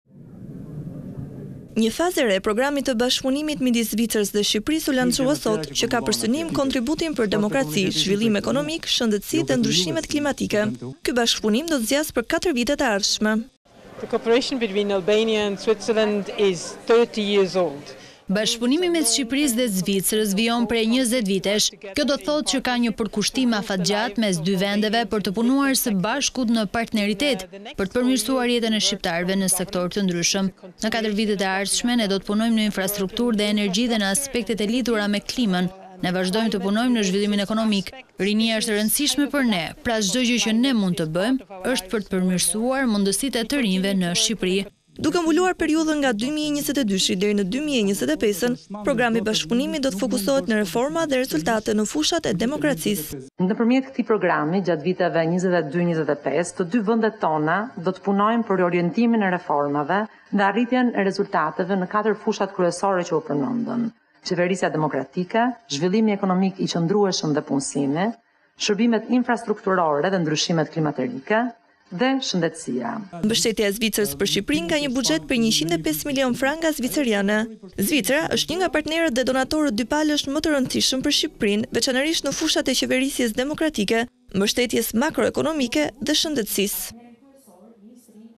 Top Channel/ Bashkëpunimi me Zvicrën. Program për ekonominë, shëndetësinë dhe klimën
Për katër vitet e ardhshme Shqipëria dhe Zvicra kanë një program bashkëpunimi që përfshin ndihmën në disa fusha.

Një fazë e re e programit të bashkëpunimit midis Zvicrës dhe Shqipërisë u lançua sot, që ka për synim kontributin për demokraci, zhvillim ekonomik, shëndetësi dhe ndryshimet klimatike. Ky bashkëpunim do të zgjasë për 4 vitet e ardhshme.

 “Bashkëpunimi mes Shqipërisë dhe Zvicrës vijon prej 20 vitesh. Kjo do të thotë që ka një përkushtim afatgjatë mes dy vendeve për të punuar së bashku në parternitet, për të përmirësuar jetën e shqiptarëve ne sektore të ndryshëm. Në 4 vitet e ardhshme ne do punojmë në infrastrukturë dhe energji dhe në aspektet e lidhura me klimën, ne vazhdojmë të punojmë në zhvillimin ekonomik, rinia është e rëndësishme për ne pra çdo gjë që ne mund të bëjmë për të përmirësuar mundësit e të rinjve ne Shqipëri, ne e mbështesim”, tha Patricia Danzi, Drejtoresha e Përgjithshme e Agjencisë Zvicerane për Zhvillim dhe Bashkëpunim.

Duke mbuluar periudhën nga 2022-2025 programi i bashkëpunimit do të fokusohet në reforma dhe rezultate në fushat e demokracisë.

 “Programi i ri i bashkëpunimit midis Zvicrës dhe Shqipërisë është një tregues tjetër e partneritetit të ngushtë midis dy vendeve tona. Shumë është arritur në katër vitet e shkuara. Jam e sigurt që shumë do të arrihet gjithashtu edhe në katër vitet e ardhshme. Programi i ri bazohet dhe vijon me mbështetje në fushat kyçe të demokracisë, ekonomisë, shërbimeve infrastrukturore dhe shëndetësisë”,tha Delina Ibrahimaj,  Ministre e Financave dhe Ekonomisë.

Mbështetja e Zvicrës për Shqipërinë  ka një buxhet prej 105 milionë franga zvicerane. Zvicra është një nga partnerët dhe donatorët dypalësh më të rëndësishëm për Shqipërinë veçanërisht në fushat e qeverisjes demokratike, mbështetjes makroekonomike dhe Shëndetësisë.